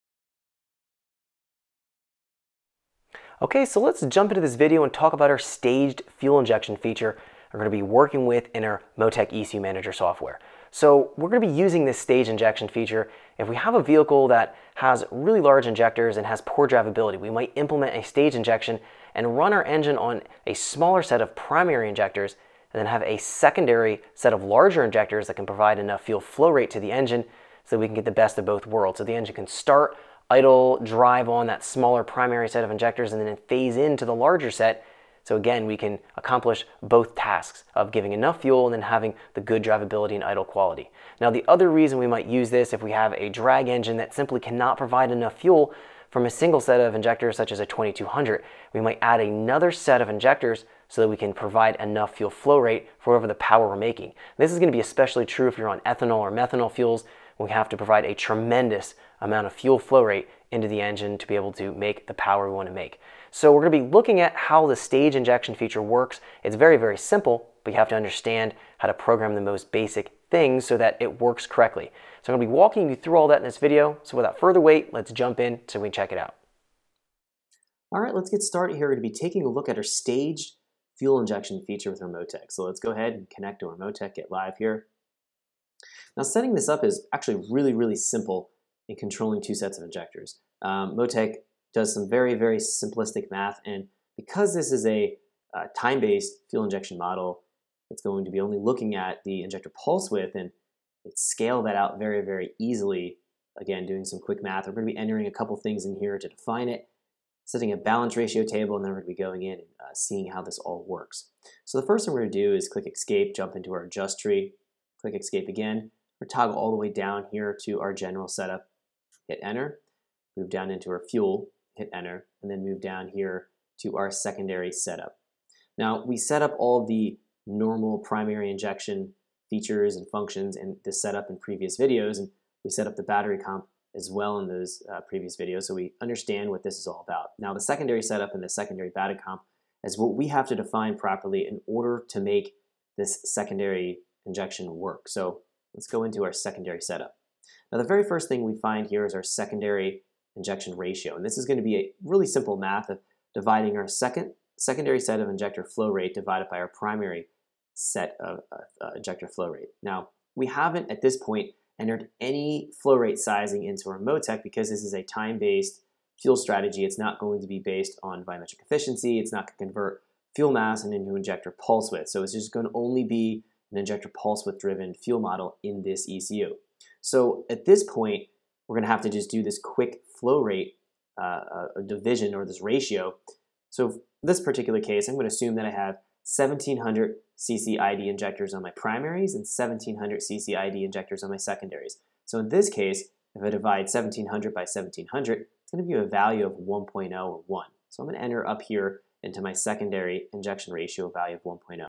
Motec: Staged Fuel Injection | Evans Performance Academy
Part 19 of the the Motec hundred series training course walks you through how to utilize the staged fuel injection feature to control a secondary set of fuel injectors.